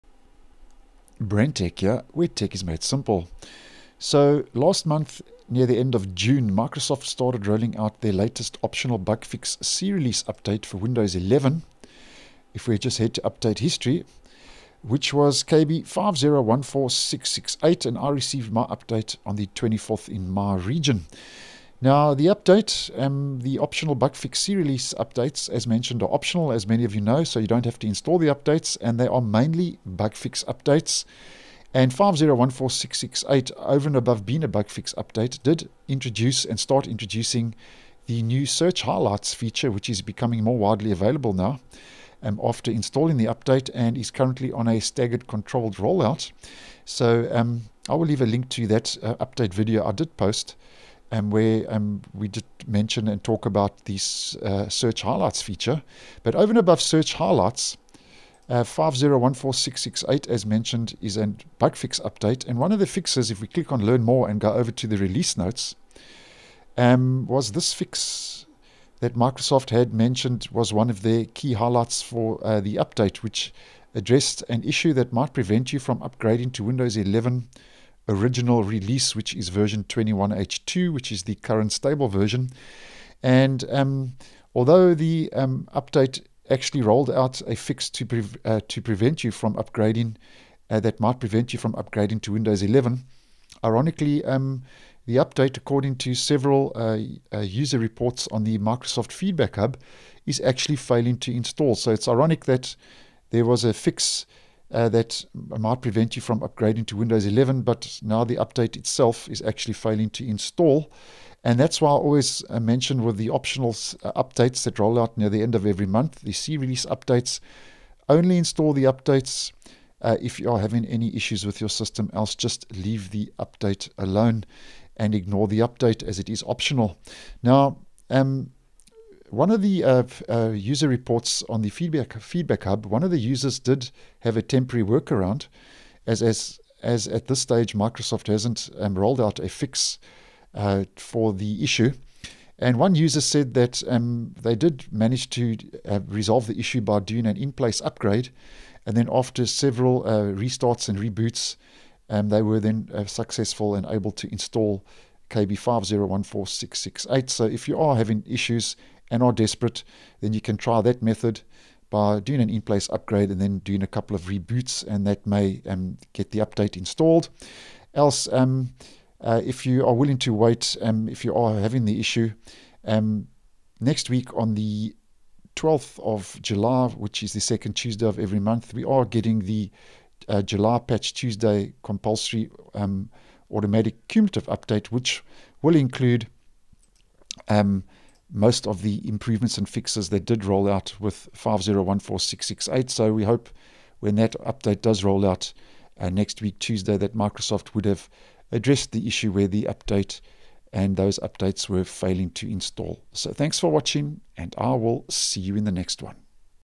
Windows 11 update KB5014668 is failing to install
The update includes the new Search Highlights feature and also addresses an issue that might prevent you from upgrading to Windows 11 (original release). According to user reports on the Feedback Hub, ironically the update itself is actually failing to install for some users.